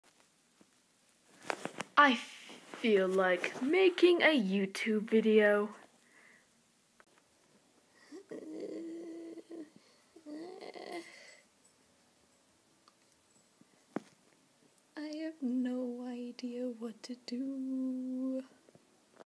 My YouTube video struggles
It's hard to draw when you are not motivated or lack creatively for the day.